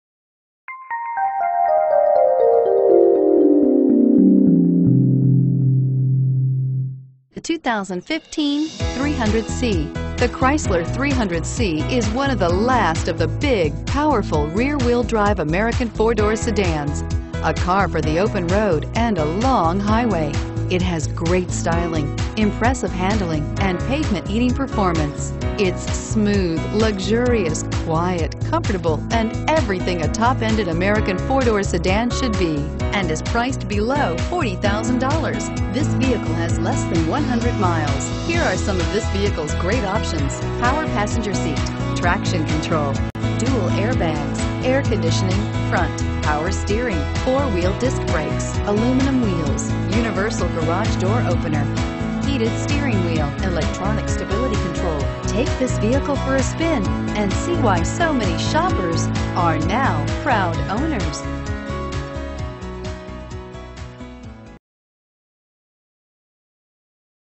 2015 Chrysler 300C Richardson TX FH842055
2015 Chrysler 300C Base

Richardson CDJR
1321 N Central Expressway

Looking for the right car? Today could be your lucky day. With 1 miles, this Black, 2015 Chrysler 300C equipped with Automatic transmission could be yours. Request more information and set up a test drive right away.